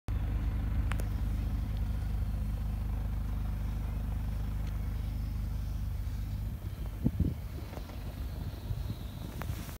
The Gornergrat Snow Monster!
Spotted by Suzanne from Matterhorn Chalets - The Gornergrat Snow Monster!

(It's actually the train clearing the tracks after the recent snow...)